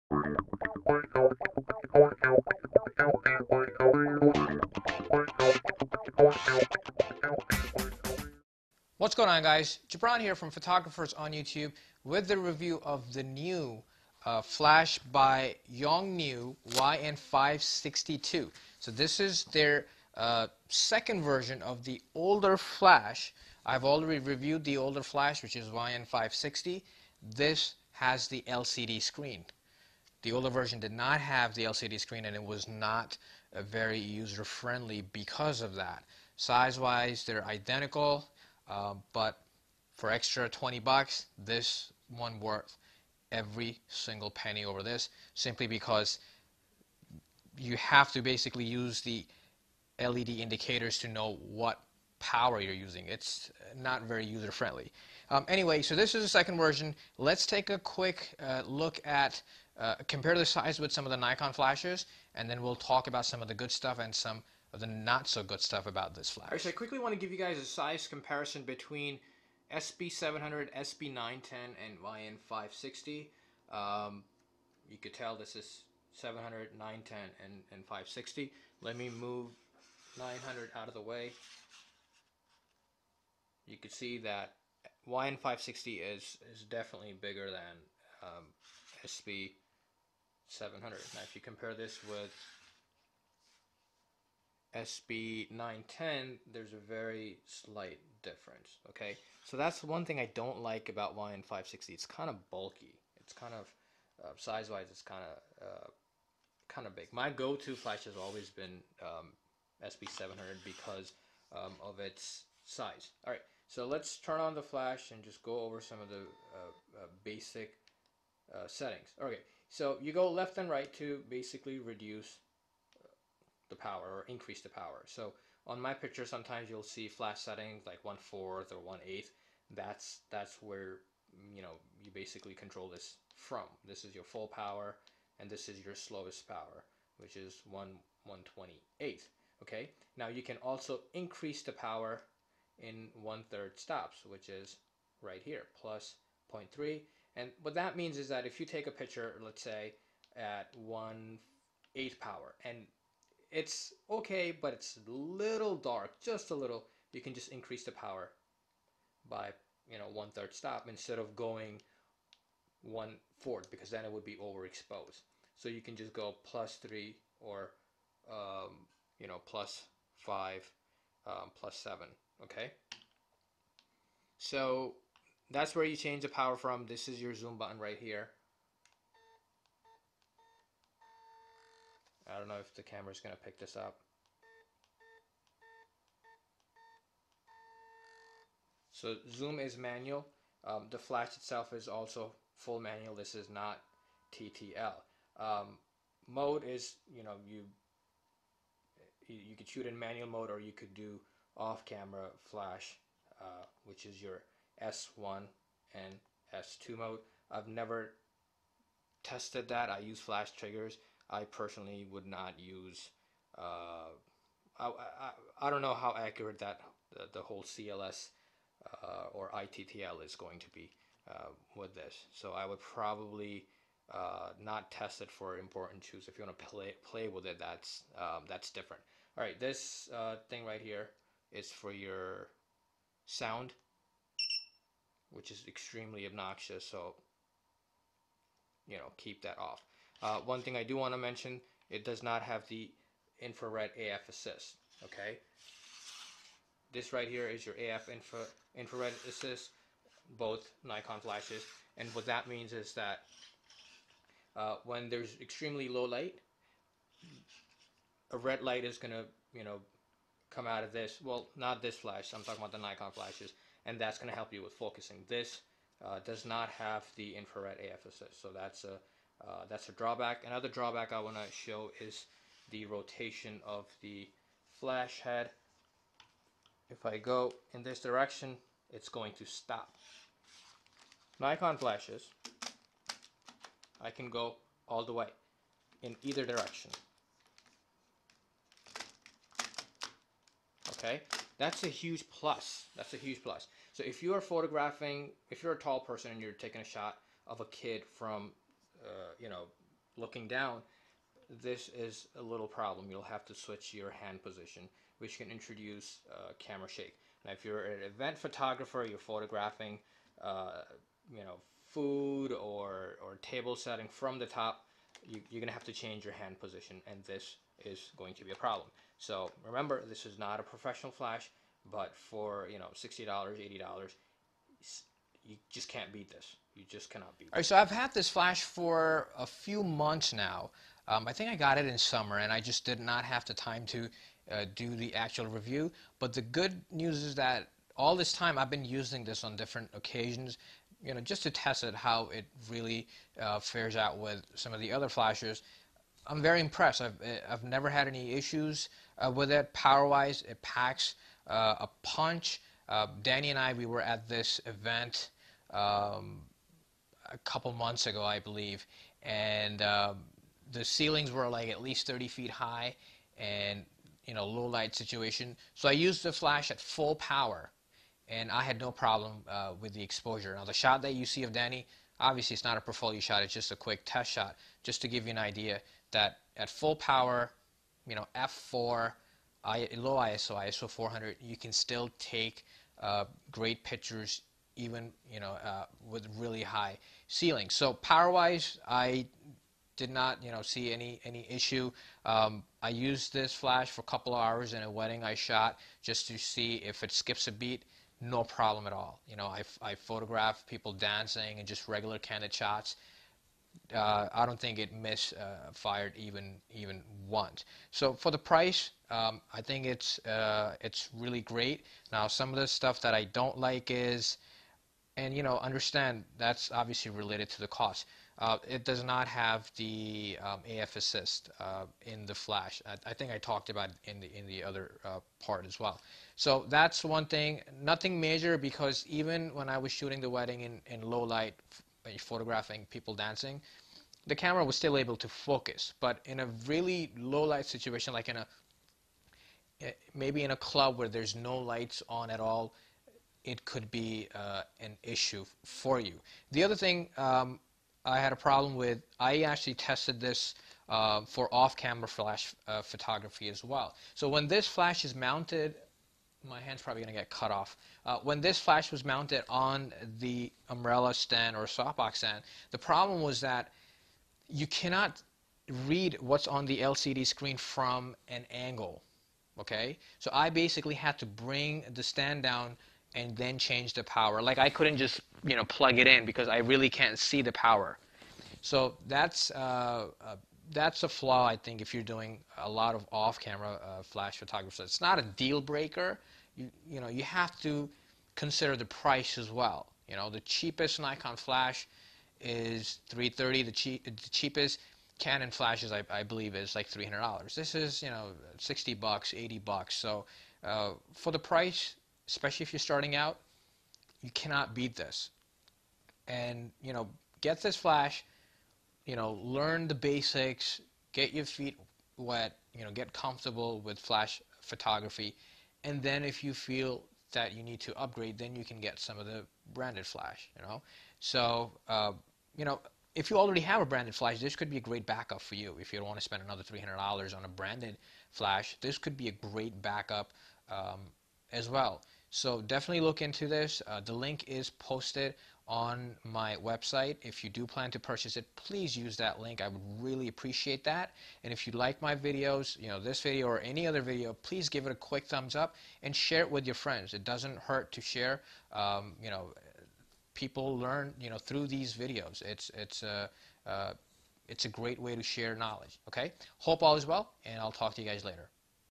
Yongnuo YN560 ii for Nikon Canon. Yongnuo YN560 II Review with Sample Pictures
Here is a review of Yongnuo YN560II flash. This is the newer version of YN560. This has the LCD screen and is much user friendly. You'll find a quick size comparison between Nikon SB700 vs Nikon SB910 vs YN560II. It's a great flash for the price. I had no major issues using it. The couple of issues I had I've discussed in the video.

Equipment Used

DSLR Nikon D3s and Nikon D600
Lenses Nikon 50mm f1.8G Nikon 24-70 f2.8
Flash Yongnuo YN560II Nikon SB700 Nikon SB910